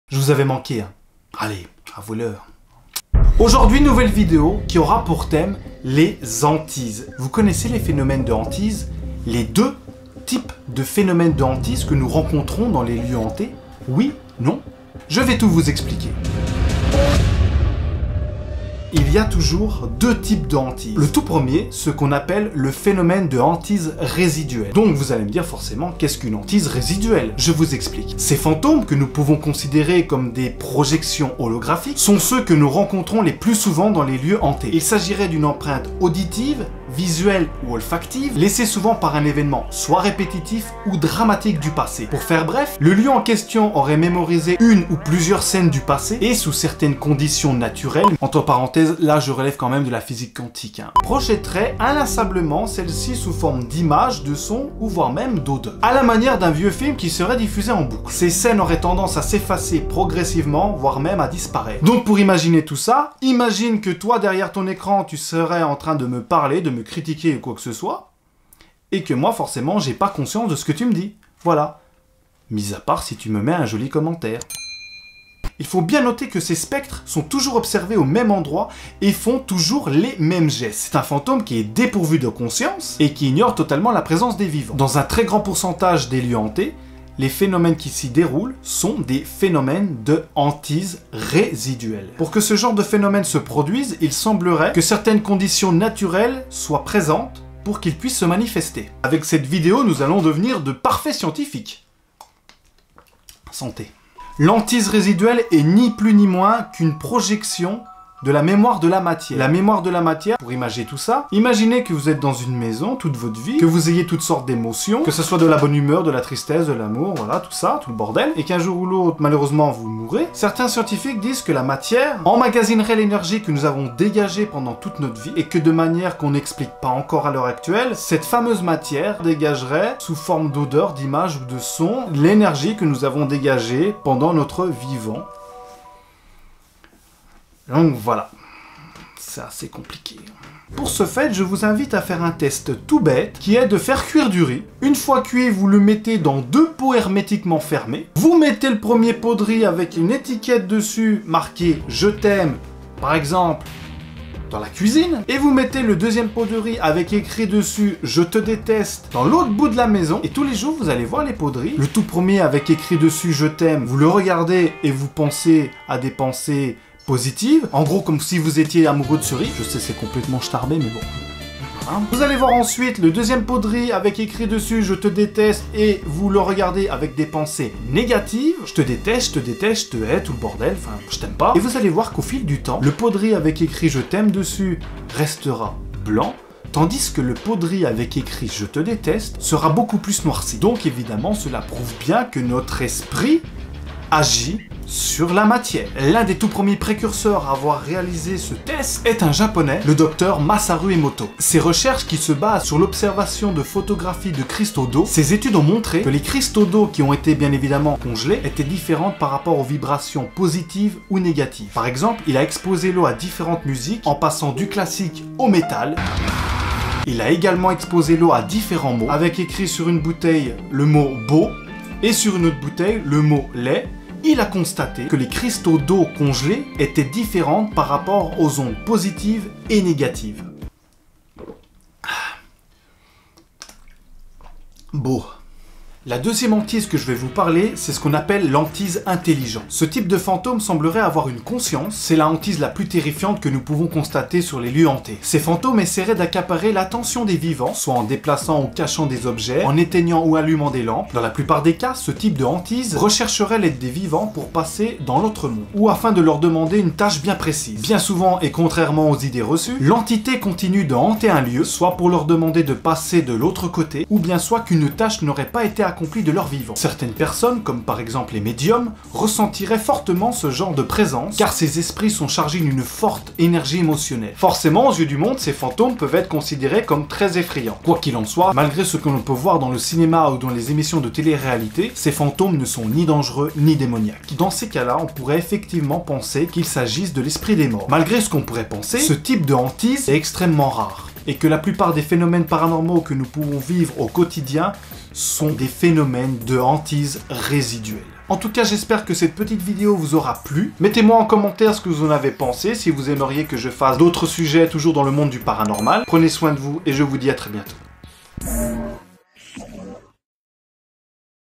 LE PHÉNOMÈNE DES HANTISES (Par Kristy Mcazzari)
Deux type de hantises existent dans les lieux dit "hanté"
L'hantise dite "RÉSIDUELLE", ainsi que l'hantise "INTELLIGENTE".
Mais savez vous la définition exacte de ces termes ?
Kristy Mcazzari vous explique tout.
Réalisation/Montage: Kristy Mcazzari
Correction: Nicolas Boularand
Une production Dreamline